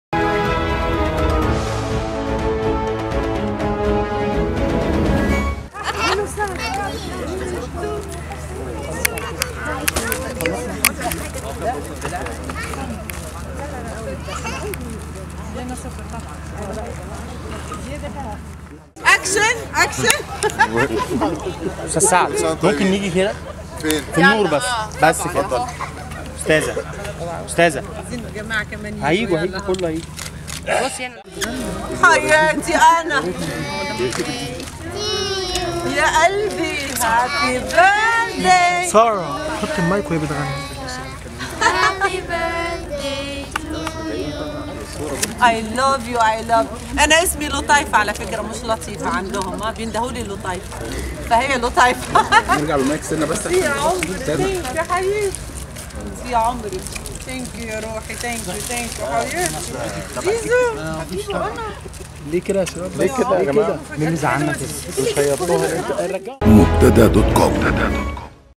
لطيفة تحتفل بعيد ميلادها
فوجئت المطربة لطيفة بحضور شقيقتها وأبنائها احتفالها، اليوم الأحد، بعيد ميلادها، فى لوكيشن تصوير مسلسل "كلمة سر"، بمنطقة 6 أكتوبر.

وحرصت لطيفة على أن يكون الحفل ضمن فريق عمل المسلسل، بحضور بعض أصدقائها، مقتصرة على حضور عدد قليل من الفنانين.

وصرحت الجهة الإنتاجية للمسلسل لحضور بعض الإعلاميين، لمدة لا تزيد عن ساعة فى وقت الراحة فقط، على الرغم من أن الشركة المنتجة كانت مشددة على عدم اقتراب الصحافة والإعلام من منطقة التصوير.